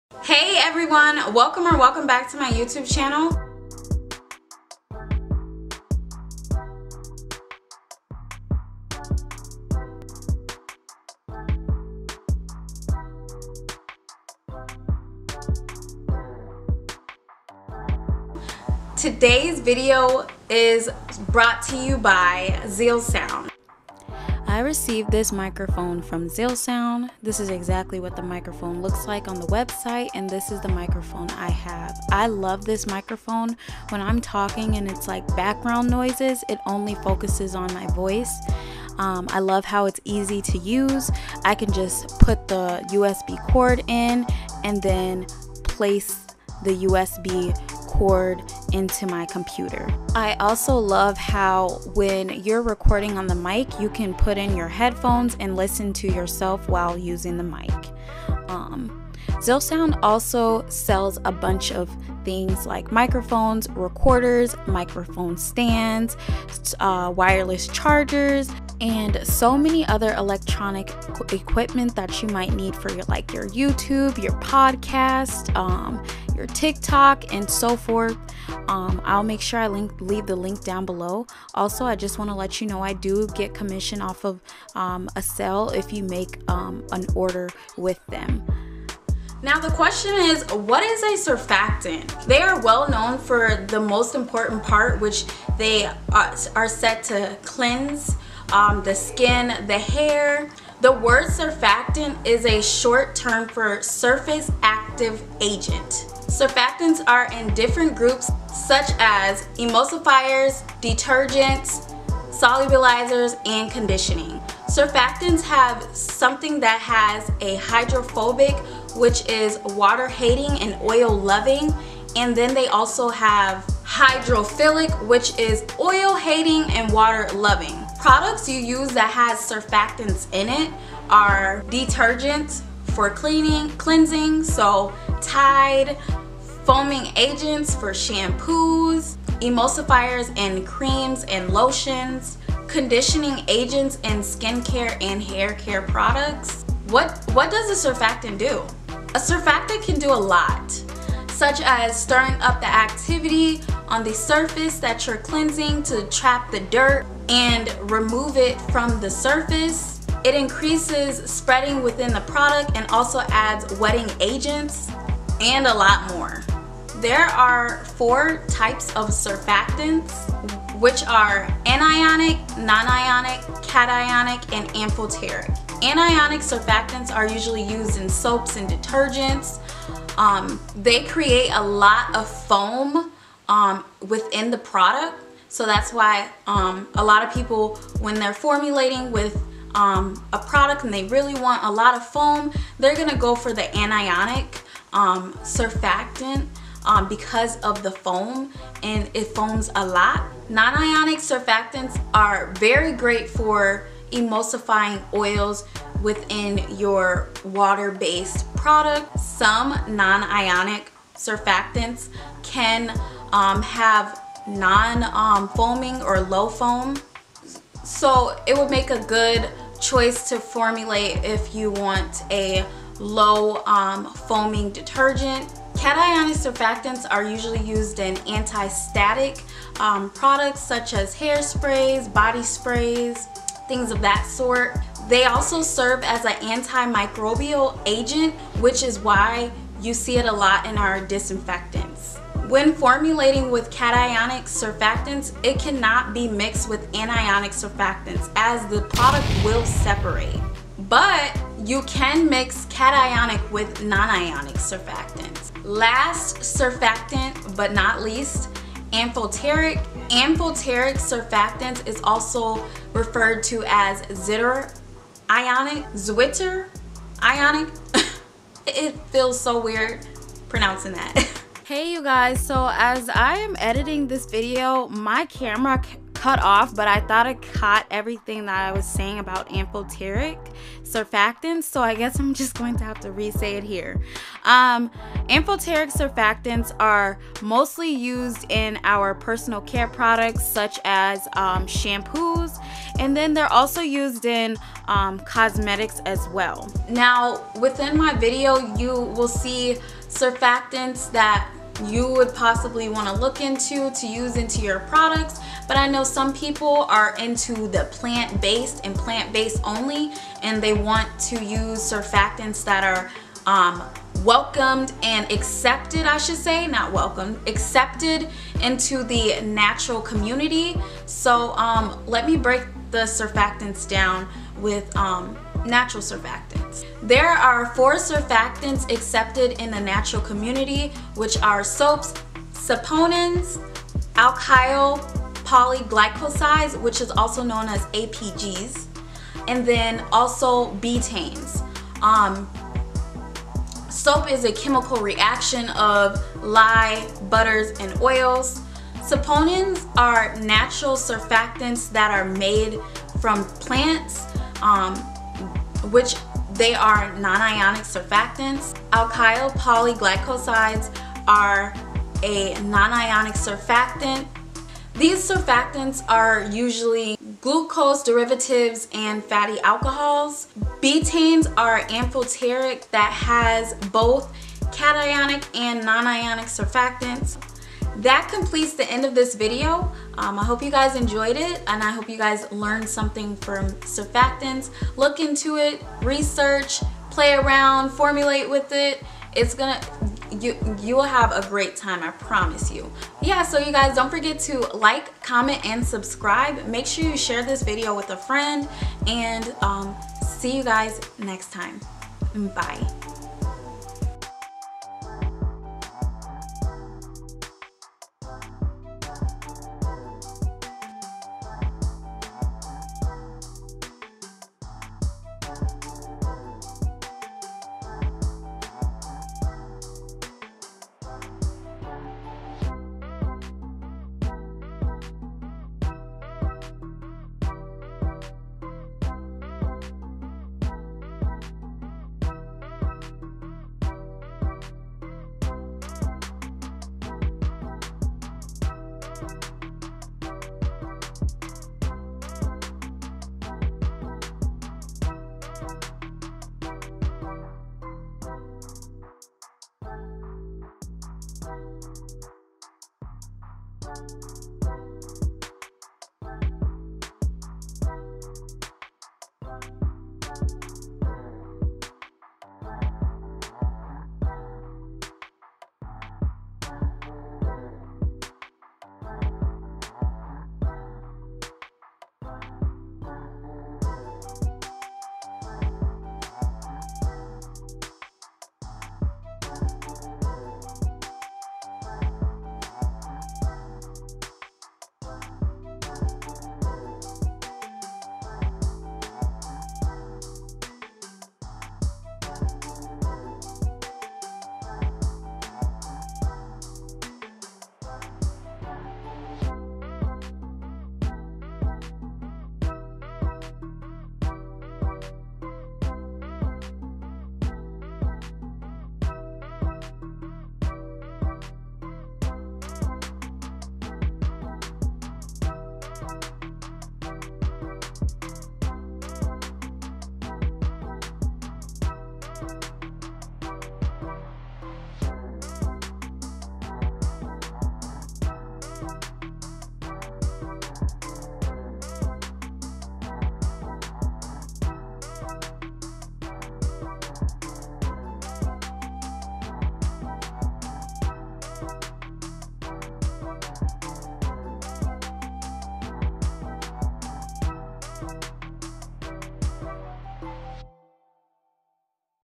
What is a Surfactant?
Hi Everyone,

Today's video is about what are surfactants, what does a surfactant do for our products and the breakdown for natural surfactants that are accepted in the natural community.

0:00 - Intro
1:29 - What is a Surfactant?
1:59 - Surfactant breakdown
2:28 - What does a surfactant do?
2:51 -  Types of Surfactants
2:55 - Anionic Surfactants
3:21 - Nonionic Surfactants
3:46 - Cationic Surfactants
4:17 - Amphoteric Surfactants
5:36 - 4 Natural Surfactants
6:27 - Outro

Thank you so much for clicking the link and purchasing! Means a lot!

6,405